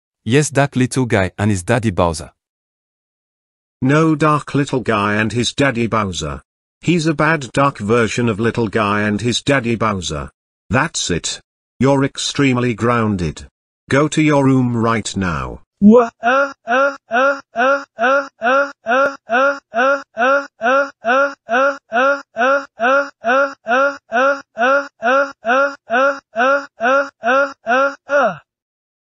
Dark Little Guy And His Daddy Bowser Jr. Says "Yes Dark Little Guy And His Daddy Bowser" / Grounded
Dark Little Guy And His Daddy Bowser Jr. says, "Yes Dark Little Guy And His Daddy Bowser" and gets grounded by Black King Bowser.